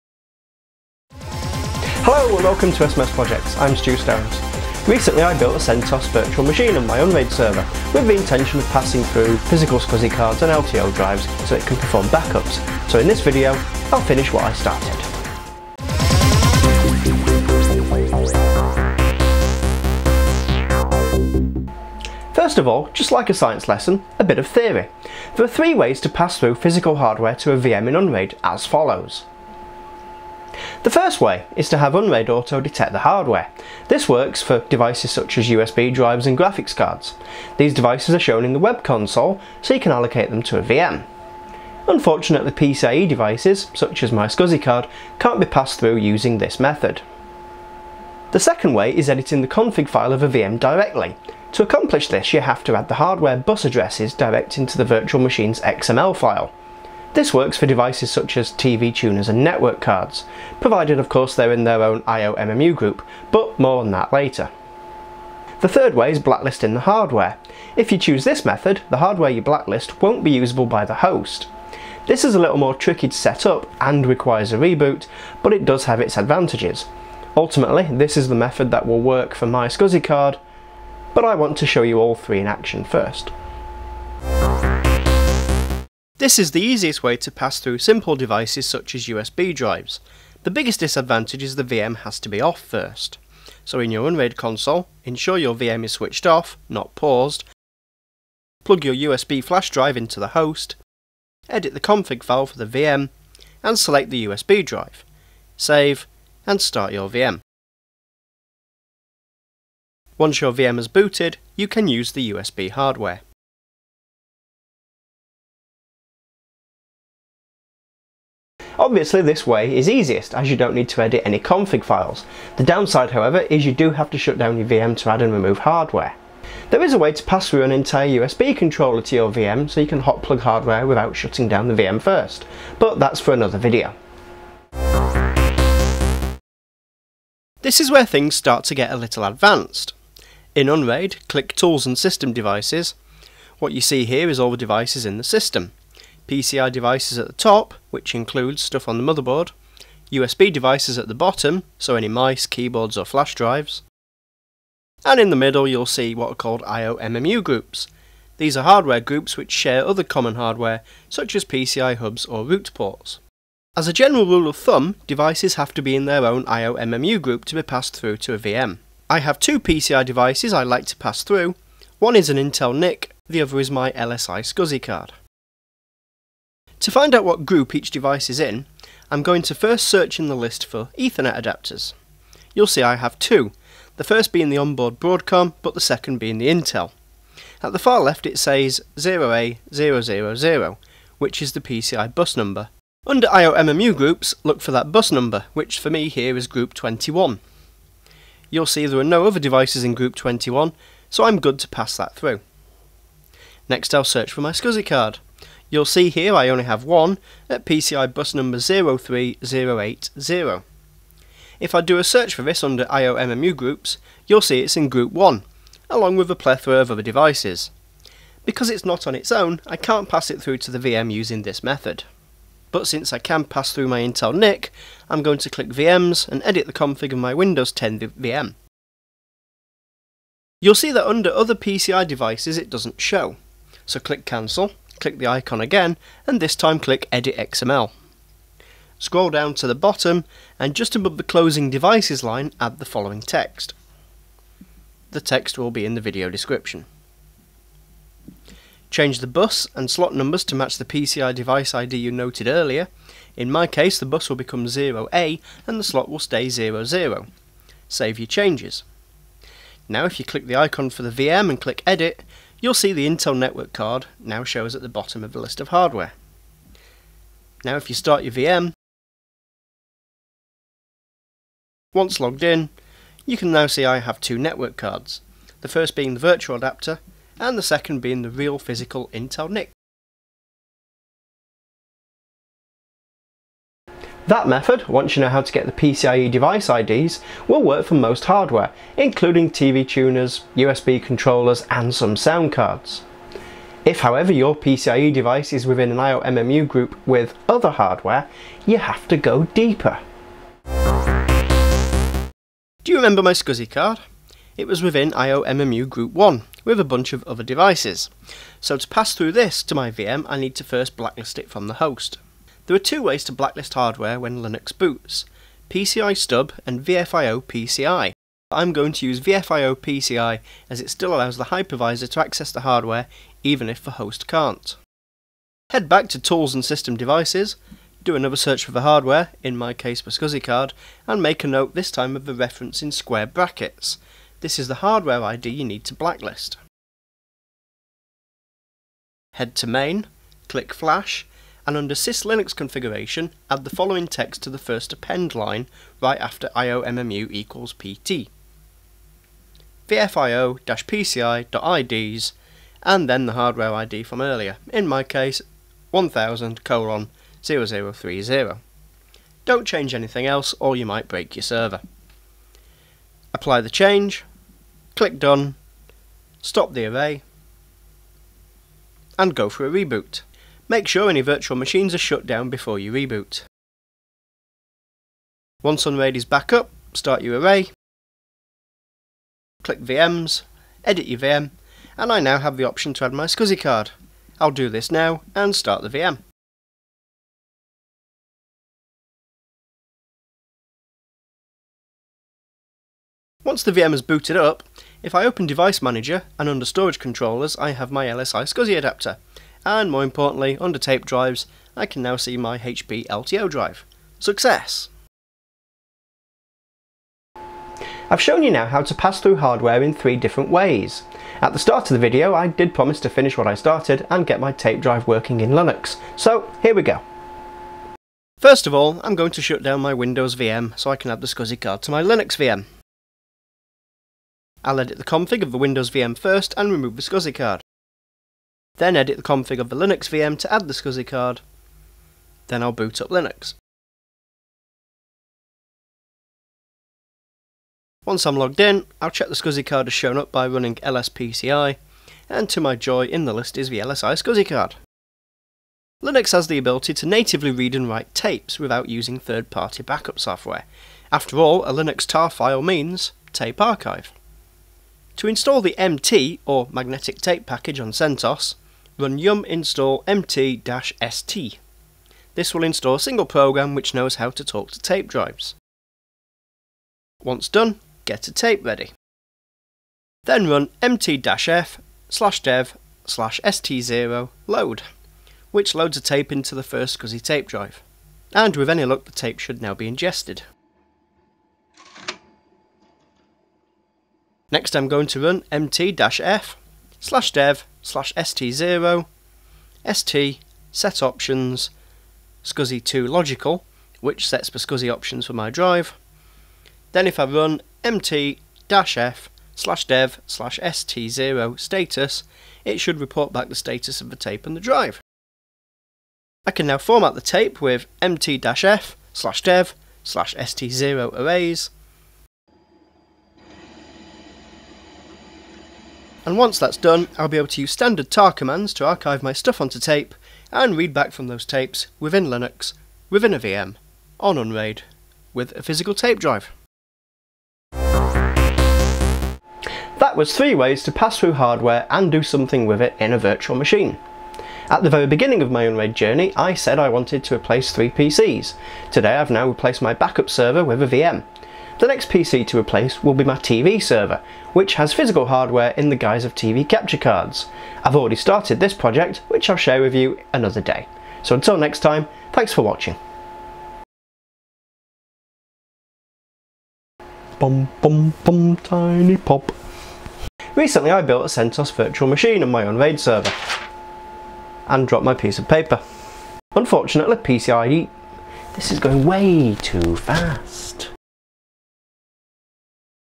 unRAID - Pass Through Hardware, 3 ways to do it
In this video I show you three ways to pass through hardware in unRAID:

1 - USB
2 - PCIe into the VM's XML
3 - Blacklist from the host

The USB method works for flash drives, keyboards, mice etc... but the VM has to be switched off before you add/remove hardware.

Editing the VM's XML works for hardware which is in it's own IOMMU group, so will work for NIC's, some sound cards, TV cards etc...

Blacklisting from the host allows you to pass-through hardware which is in a shared IOMMU group, such as my SCSI card.  Because it has a SCSI BIOS, it is grabbed by the hardware earlier in the boot, and can't be separated into it's own group.  This method, even though it's still in a shared IOMMU group, prohibits the host from seeing the card, and allowing it to pass-through to the VM.

Using the Blacklist method, I got my SCSI card and LTO drive working from within a VM - which I think is cool!

for my blog post on this video.

I want to make a huge shout out to the community on the Lime Technology forums, who's knowledge really really helped when researching this video.